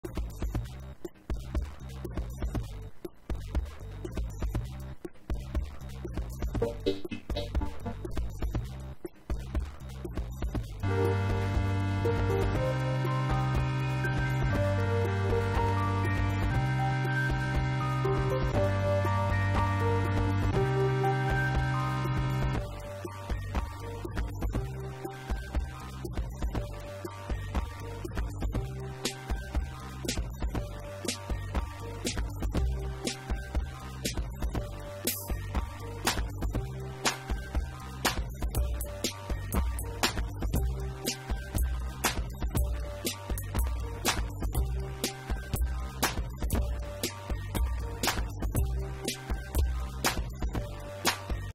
Omnichord OM-84 Loop
Just got this thing yesterday and I love the sound. Its a little glitchy but the electronics inside don't look so esoteric. Return it and bid on a new (old) one or keep it, touch up solder joints and hope?